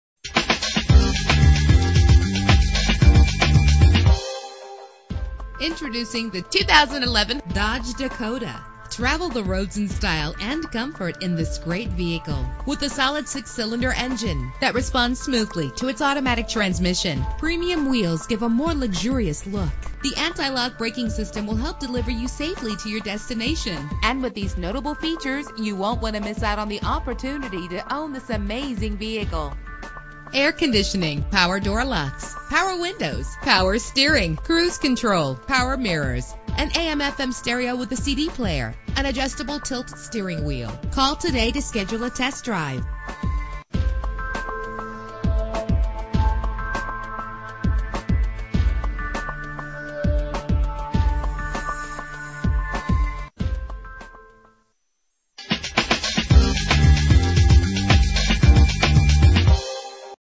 2011 Dodge Dakota - Columbia IL
Year: 2011
Make: Dodge
Model: Dakota
Trim: Big Horn/Lone Star
Engine: 3.7 liter 6 cylinder 12 valve
Transmission: Automatic
Color: Hunter Green Pearlcoat
Mileage: 0
Address:
500 Admiral Weinel Blvd